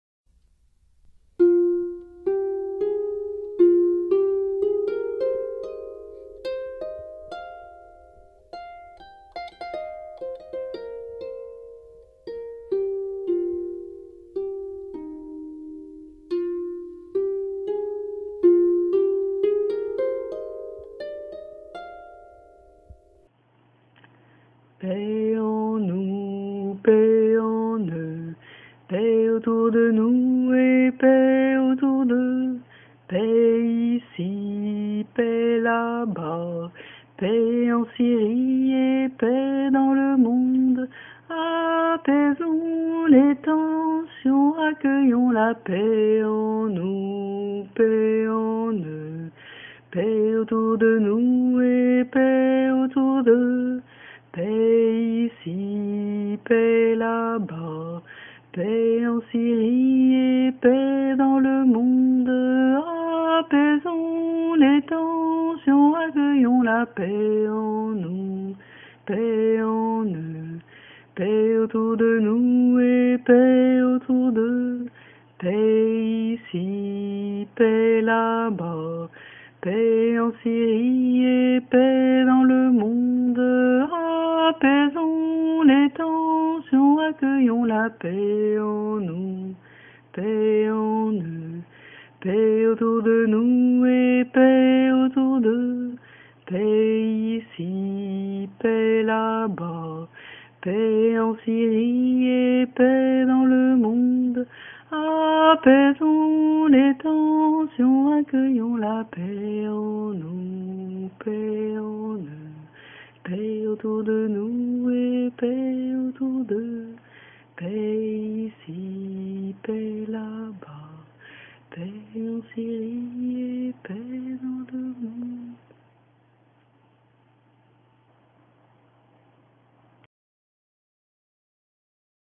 rituel pour la paix
rituel pour la paix et les réfugiés de guerre du Proche-Orient

Au sein de la Ligue Wiccane Éclectique, plusieurs personnes ont évoqué leur tristesse et leur sentiment d'impuissance face aux tragédies de notre époque , particulièrement en ce qui concerne le Proche-Orient. Nous avons décidé d'agir en organisant une convergence rituelle pour la paix et les réfugiés de guerre du Proche-Orient. Nous vous proposons de ritualiser ensemble avec une base rituelle commune.

*    Mercredi 11/11/2015 à 21h (heure d’hiver de Paris, GMT+2),

*    allumer une bougie blanche,

*    diriger ses pensées de réconfort et de paix vers les peuples en souffrances et les réfugiés de guerre du Proche-Orient, avec un geste d'embrassement, en serrant contre son cœur,

*    adressez-vous à une divinité avec laquelle vous avez une affinité, qu'il vous semble approprié d'associer à ce rituel, adressez-lui une prière et demandez-lui son aide,

*    chantez ou répétez :
    Paix en nous, paix en eux,
    Paix autour de nous et paix autour d'eux,
    Paix ici, paix là-bas,
    Paix en Syrie et paix dans le monde,
    Apaisons les tensions, accueillons la ... [reprendre au début, répéter l'ensemble ad libitum]

Quelques suggestions (facultatif) :
- utiliser la forme rituelle dont vous avez l'habitude : tracer un cercle, appeler les éléments etc.
- se tourner en direction du Proche-Orient
- faire une offrande à la divinité.
- enrichir votre autel avec d'autres symboles, d’autres bougies, couleurs, utiliser de l'encens etc.
- écrire la formule, utiliser des instruments de percussions pour accompagner le chant, danser etc.
- faire un rituel en groupe, le répéter sur une période etc.

Soyons nombreux à envoyer ce soir là amour et guérison vers tous ceux qui sont dans cette détresse.
Que vous offriez cette prière seul et dans la simplicité, ou décidiez d'organiser quelque chose de plus élaboré en groupe, votre aide est toujours aussi précieuse !